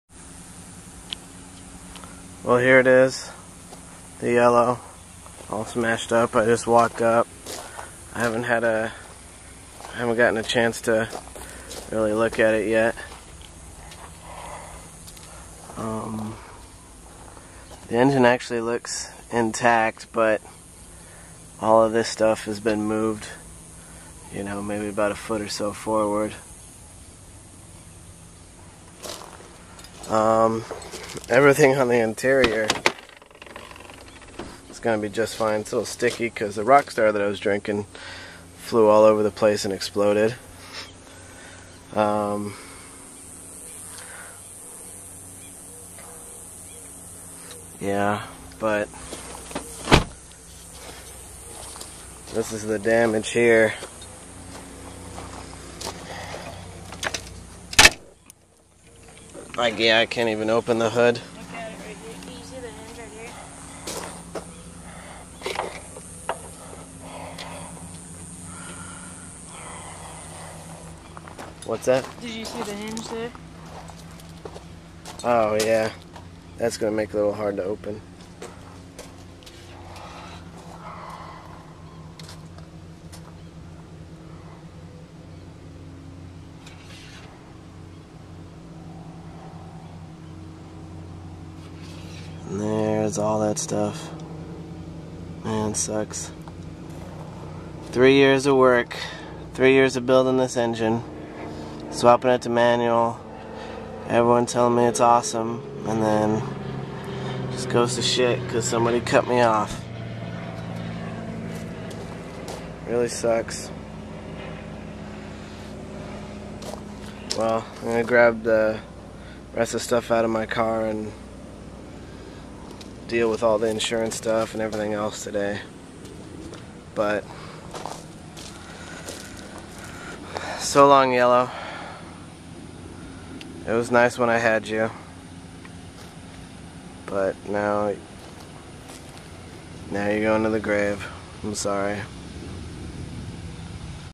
RIP The Yellow
1995- 8/15/2010

So long yellow. You will be missed.

This is the yellow at the police lot the day after the crash.

Since people are ignorant: I did not wreck the car. A fucking bitch on the highway changed lanes into me hit the fender and caused my car to spin and hit a guard rail.

And I have a new yellow now with the engine and transmission from this car.